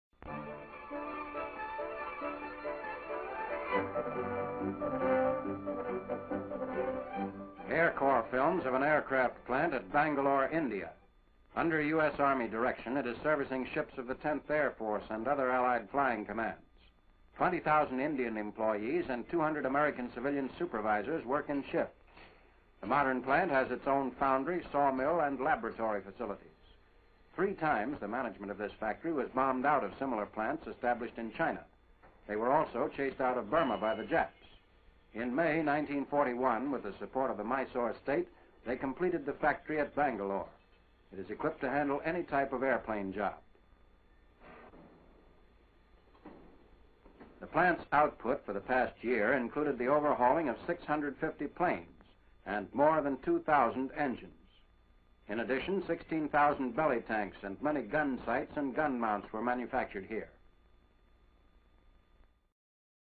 American Aircraft being overhauled at India's Hindustan Aircraft factory during World War 2
Precursor to the  Hindustan Aeronautics Limited [HAL], the  Hindustan Aircraft [HA] factory setup by the king of Mysore in the Indian city of Bengaluru [then, Bangalore] was used for servicing & overhauling of aircrafts belonging to the Allied Forces, that included the U.S.A during World War II

Video courtesy: Critical Past

Moi Blog: AA Me, IN